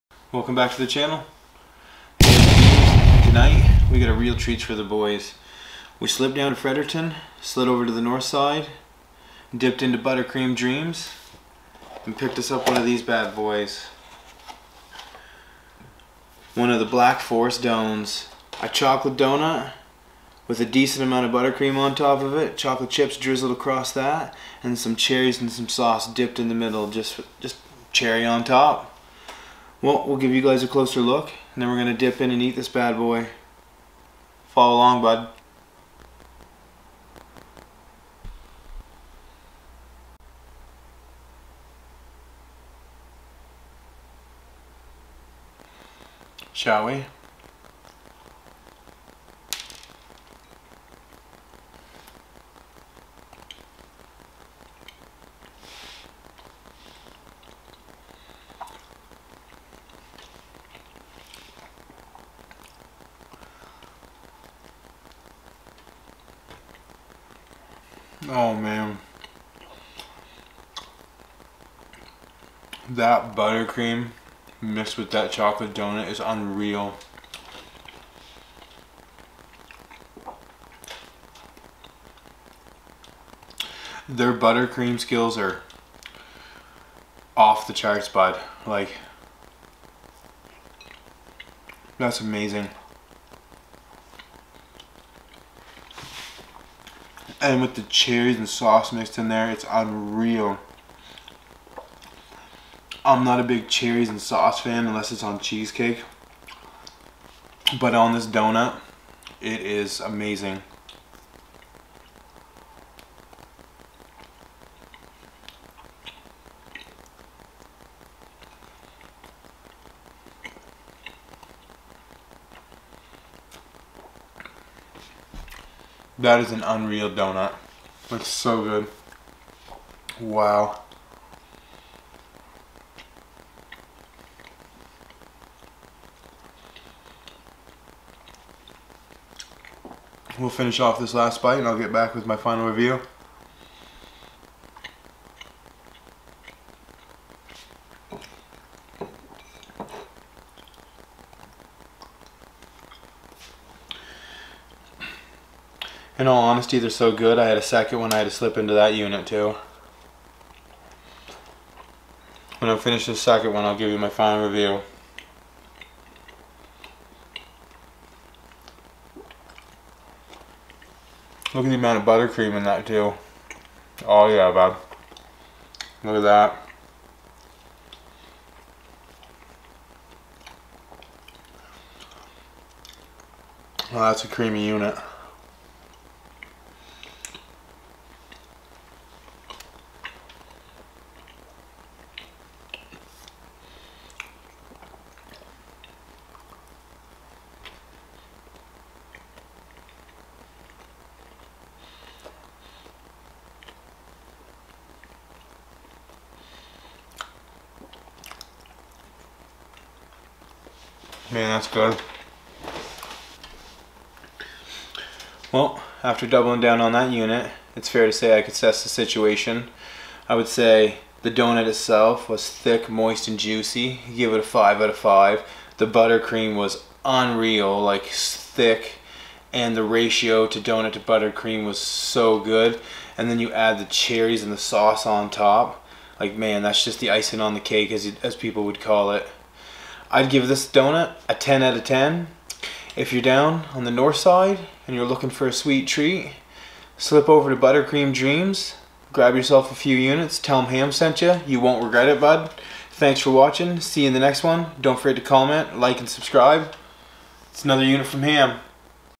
Treats For The Boys | A Deliciously Moist Review Of The Black Forest Done | A Buttercream Treat!
A delicious little review of the black forest dones, a delicious buttercream donut from BUTTERCREAM DREAMS in Fredericton N.B.
Watch me taste this gnarly creation and give an excellent ham style review.
Come follow along on another view from the East Coasts Finest.